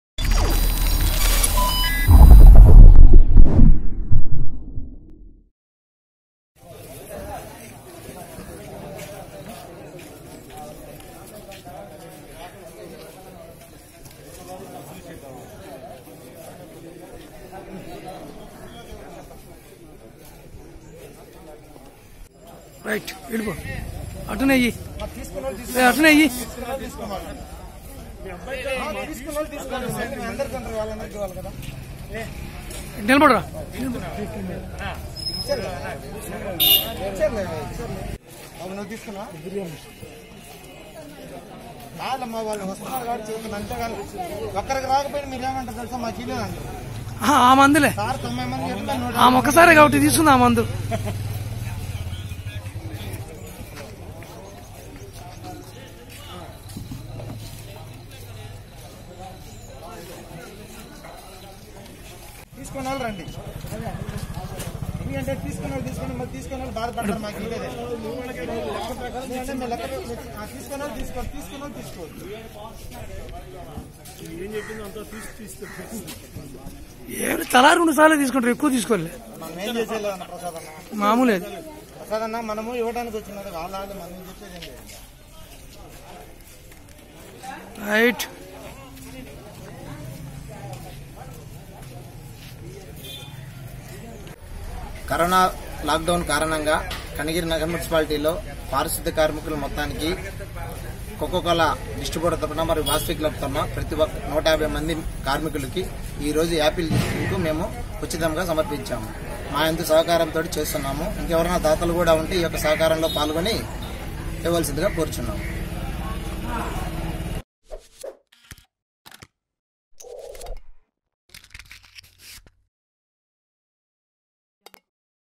కనిగిరి లోని పారిశుధ్య కార్మికులకు ఎనర్జీ డ్రింక్స్ వాసవి క్లబ్ వారి ఆధ్వర్యంలో పంపిణీ ||VS9NEWS
Watch:
కనిగిరి లోని మున్సిపల్  పారిశుధ్య కార్మికులకు ఎనర్జీ డ్రింక్స్, ఆపిల్ జ్యూస్ ని థమ్స్ అప్ రమేష్, వాసవి క్లబ్ వారి ఆధ్వర్యంలో పంపిణీ చేయటం జరింగింది.